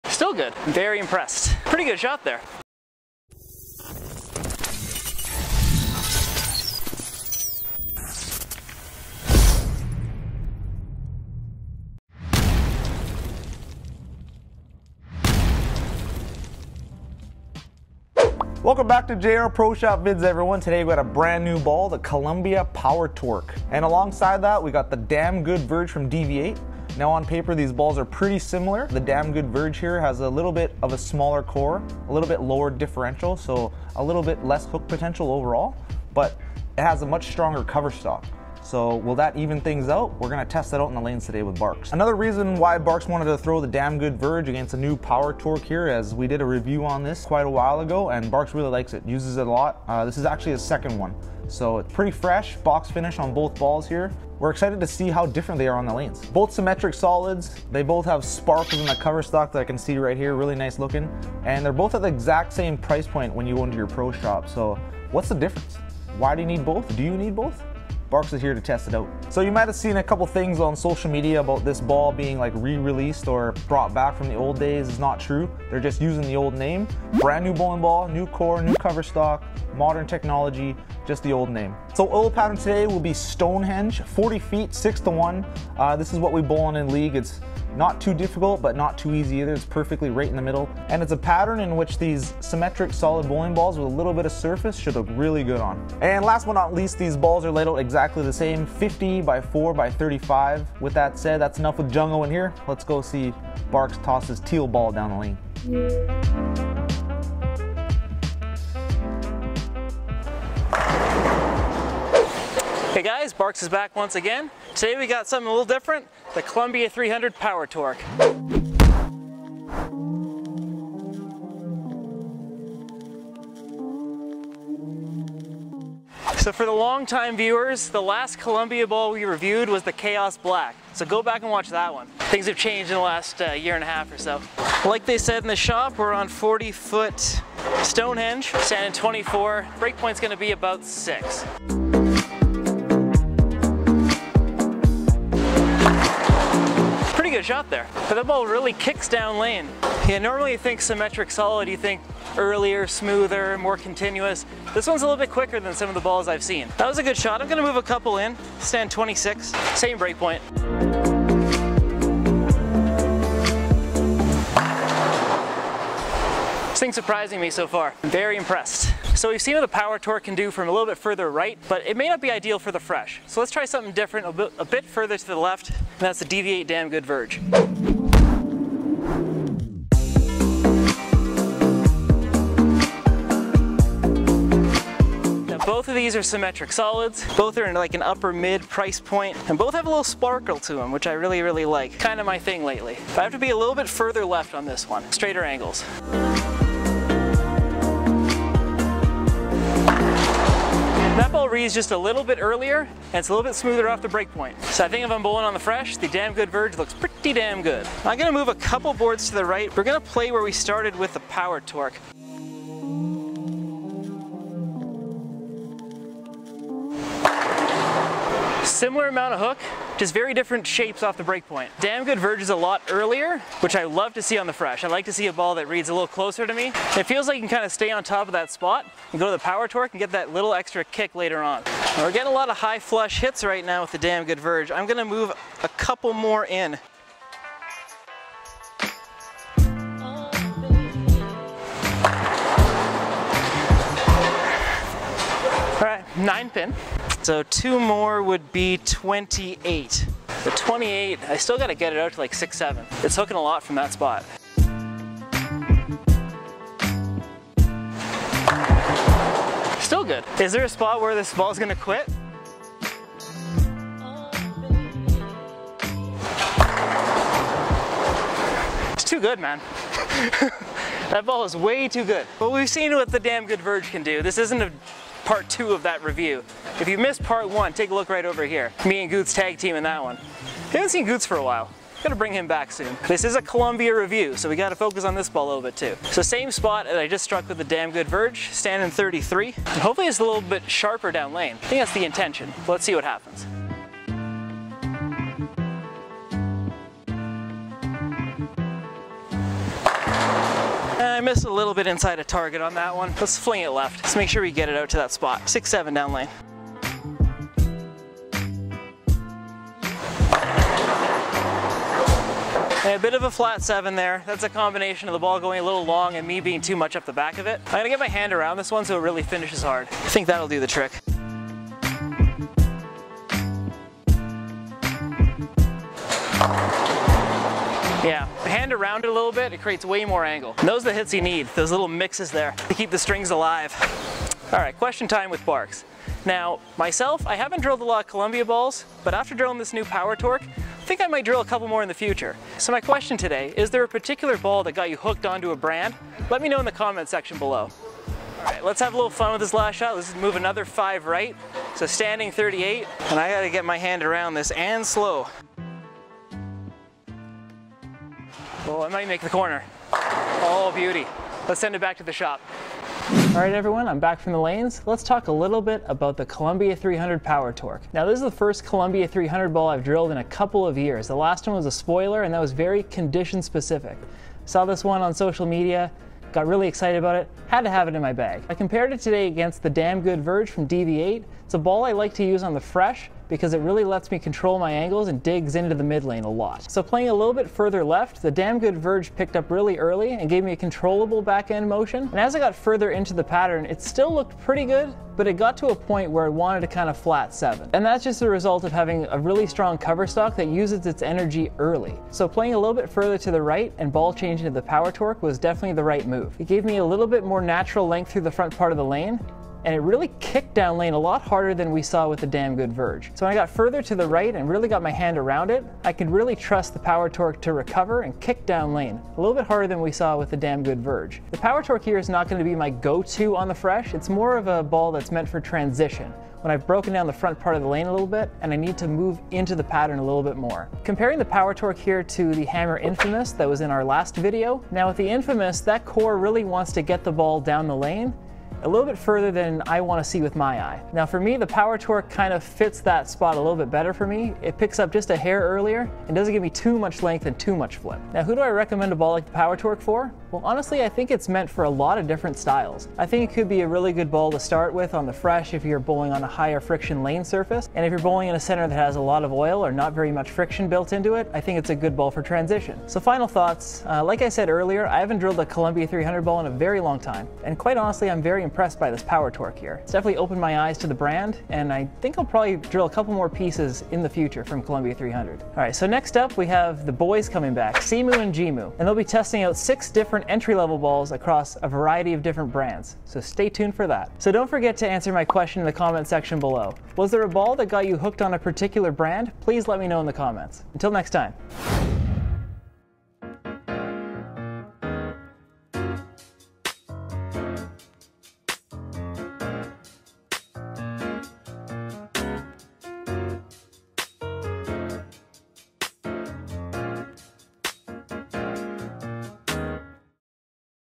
Columbia 300 Power Torq | Bowling Ball Review | OLD NAME BETTER BALL??
JR Pro Shop Vids has not reviewed a ball from Columbia 300 since the Chaos Black (our first video). Barks is back ready to throw the new Power Torq vs the Damn Good Verge.

WHAT IS JR PRO SHOP???

Timestamps!!!

0:00 - Intro
1:52 - Bowling
6:03 - Summary